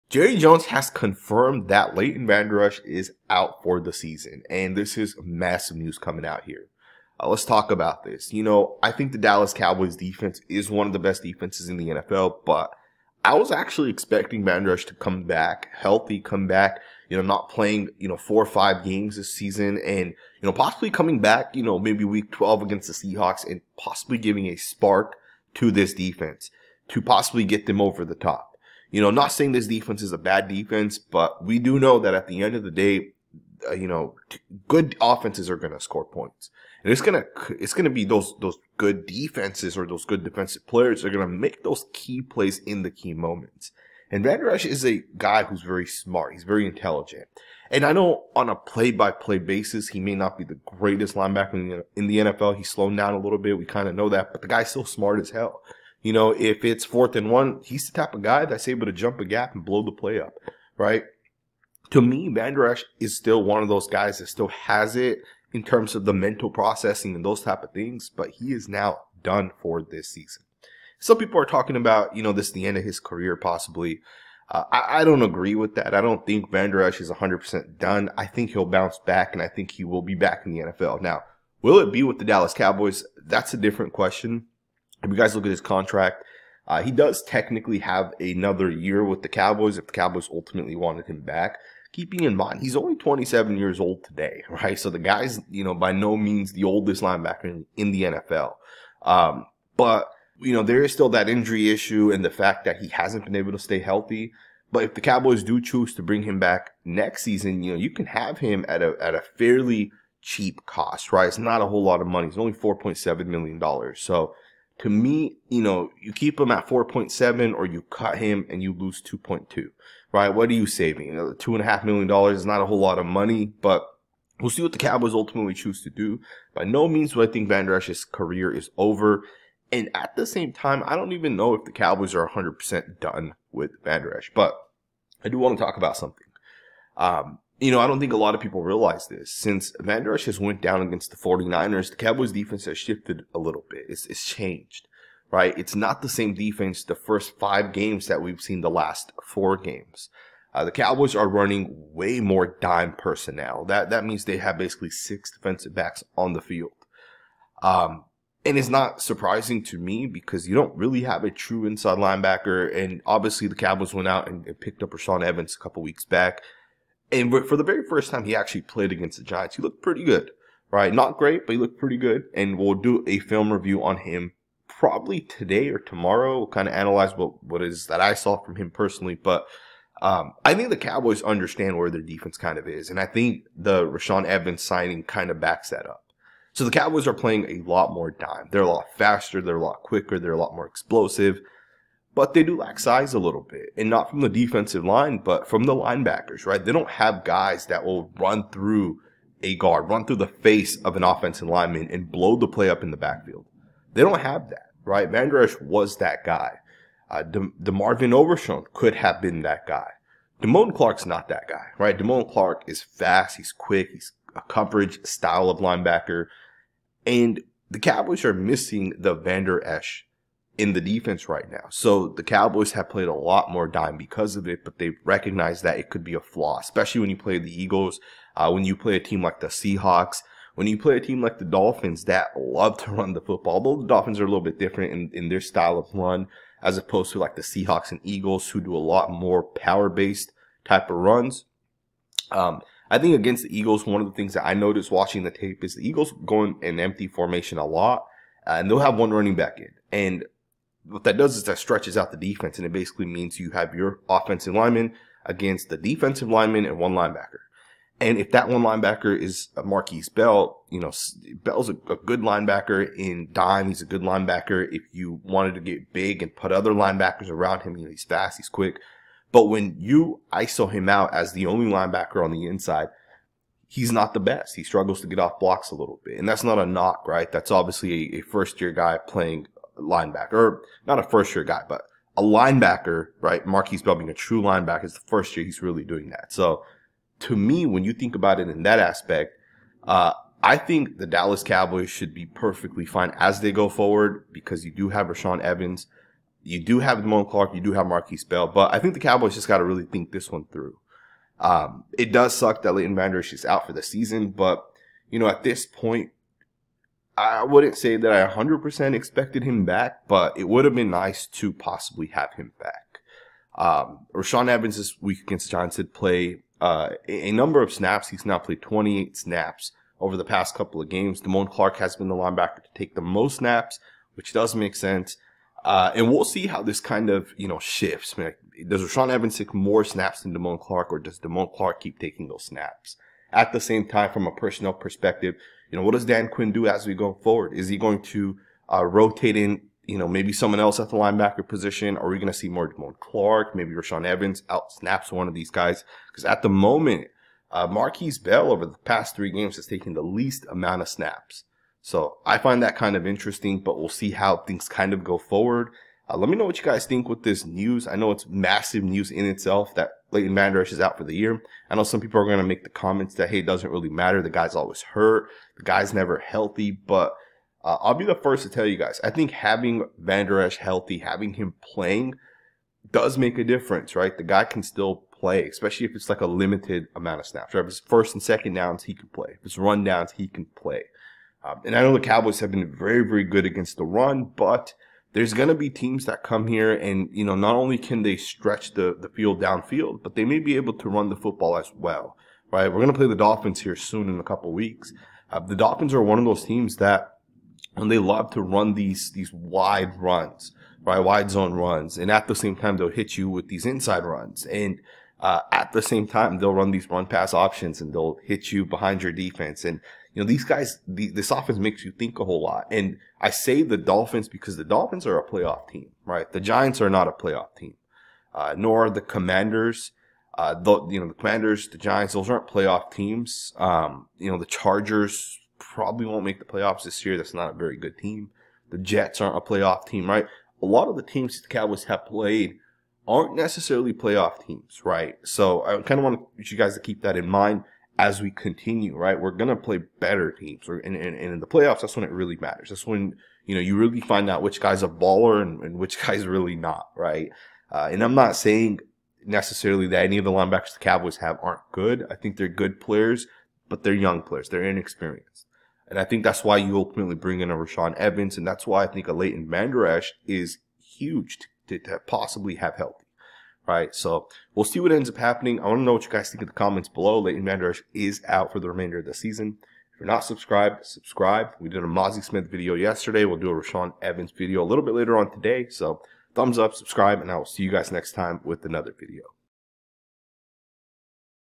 BREAKING: Cowboys Leighton Vander Esch is OUT FOR SEASON!
The Dallas Cowboys LB Leighton Vander Esch is out for the year. Lets discuss it.

Cowboys report, Dak Prescott, Dan Quinn, Mike McCarthy